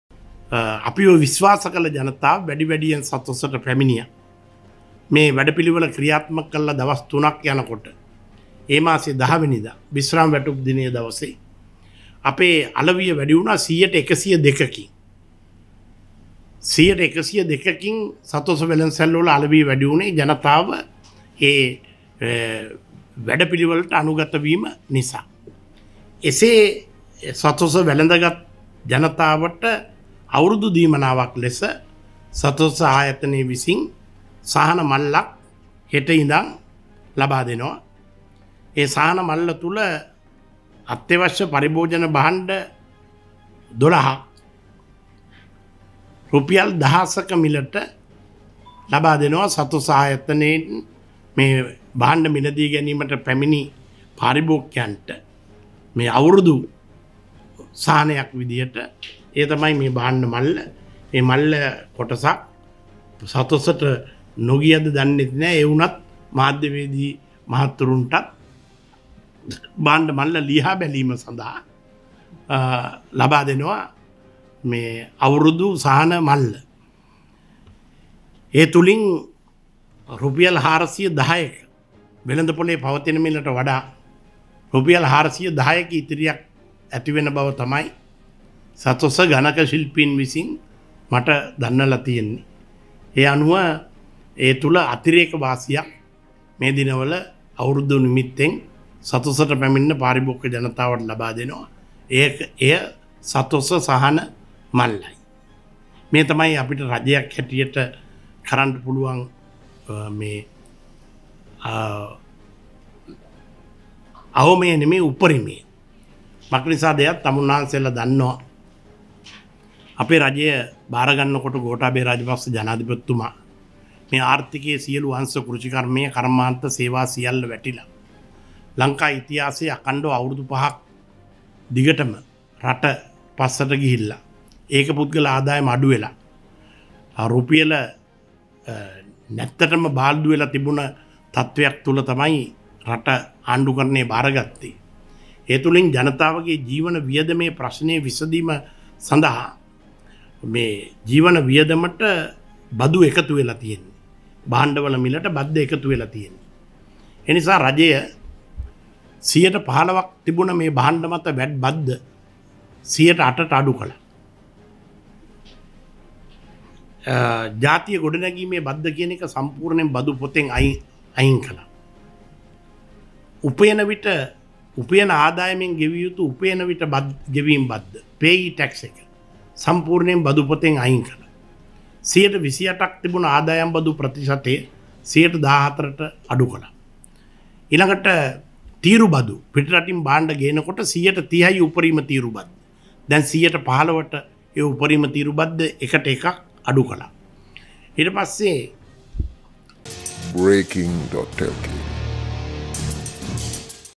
ආණ්ඩුවෙන් කරන්න පුළුවන් උපරිමය ලෙස අවුරුද්දට සතොසෙන් සහන මල්ලක් | Breaking.lk
ආණ්ඩුවෙන් කරන්න පුළුවන් උපරිමය ලෙස අවුරුද්දට සතොසෙන් සහන මල්ලක්